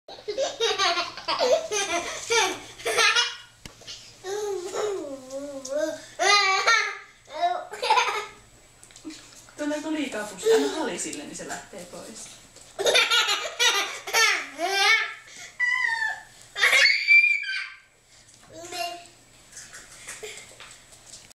Phönix Shila Van Youngmuskyteira Alias Flow & das Baby..flv
Wenn Gewissenhaft sozialisierte Kooikerhondje-Puppys gewissenhaft mit Kleinkinder zusammengeführt werden, ist die Zukunft gesichert!!
gewissenhafte Kooikergrüsse Susanne unddie Kooikerhondje's